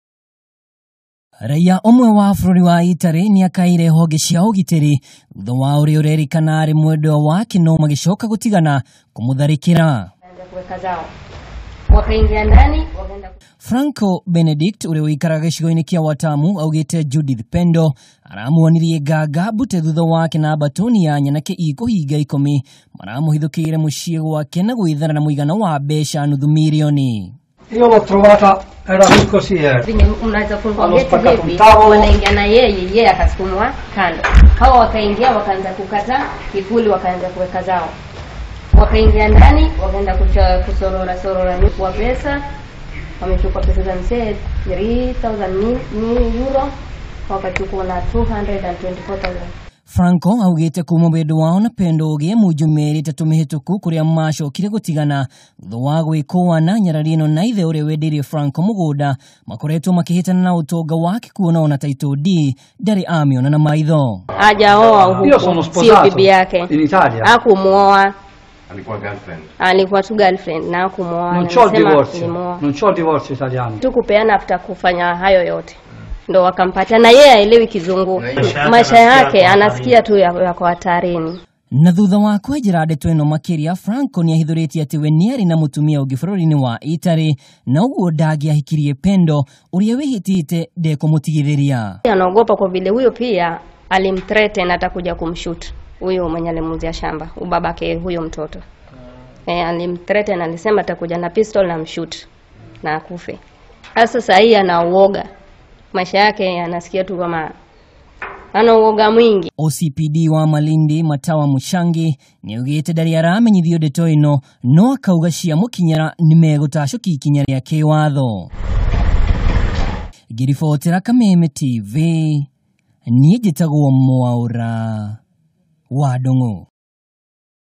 Raiya kuuma bururi wa Italy gutharikirwo ni mwendwa wake Watamu
Raiya umwe wa Italy uikaine  ta Franco Benendicto niatungitio ruo ni mwendwa wake wa tene uikaine ta Pendo uria uramutharikiire gwake mucii ari na mbatuni ya nyanake na kumuiya ciringi ngiri  magana matano.
Kurerikana aya eeri matiganire mieri itatu mihituku thutha wa gwikuana, no Pendo unyitaniri-ini na ithe mumuciari  ndaigana kumutigithiria,mworoto wake ari gutigirira Franco niahara.

Mwaura wa Ndung'u na uhoro makiria